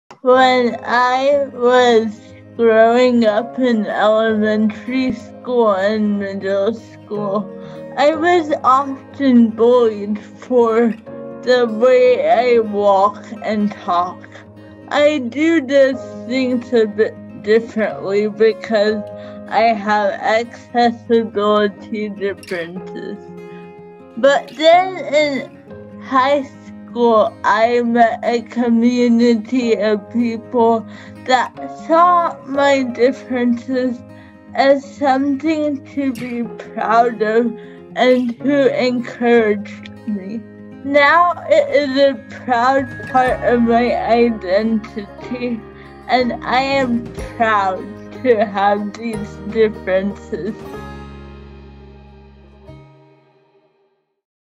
Special Needs Special People: Inspiration for the program
It wasn’t until high school that UNC student Amrutha Nandam met classmates who helped her feel proud about being herself and her accessibility differences. The experience inspired her to launch Special Needs Special People in 10th grade to spread awareness and promote inclusion.

“That really had a strong impact on how I viewed my own accessibility differences,” says Nandam. “I want to create the same inclusive environment that I had in high school for children of all abilities.”